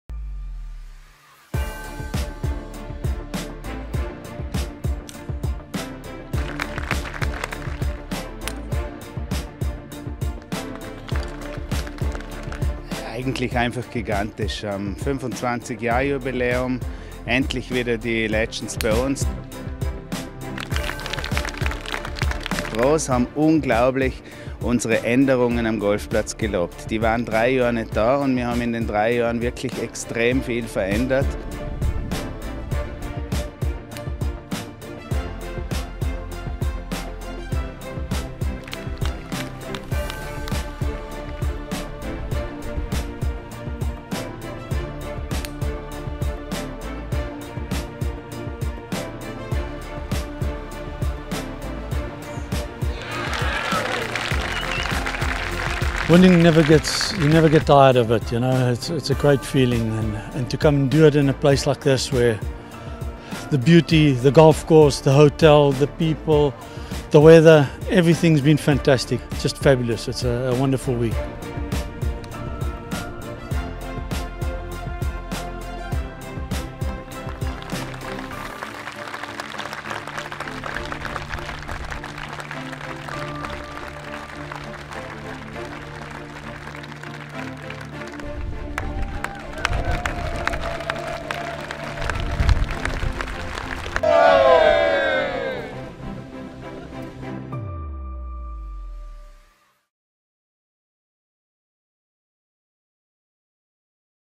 SSO 2022 | Tag 3
Weltklasse Golf in Bad Ragaz
Das Swiss Seniors Open ist das älteste Turnier der Legends Tour und wird seit 1997 jährlich auf dem 18-Loch Championship Course des Golf Club Bad Ragaz durchgeführt.

Film: Uwe Bornemeier